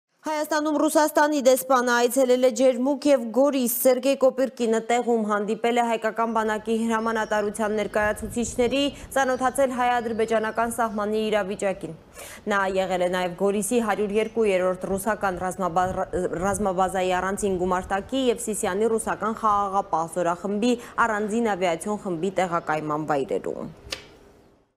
Հայաստանում Ռուսաստանի դեսպանն այցելել է Ջերմուկ և Գորիս
Հայաստանում Ռուսաստանի դեսպանն այցելել է Ջերմուկ և Գորիս: Սերգեյ Կոպիրկինը տեղում հանդիպել է հայկական բանակի հրամանատարության ներկայացուցիչների, ծանոթացել հայ-ադրբեջանական սահմանին տիրող իրավիճակին: Նա եղել է նաև Գորիսի 102-րդ ռուսական ռազմաբազայի առանձին գումարտակի և Սիսիանի ռուսական խաղաղապահ զորախմբի առանձին ավիացիոն խմբի տեղակայման վայրերում։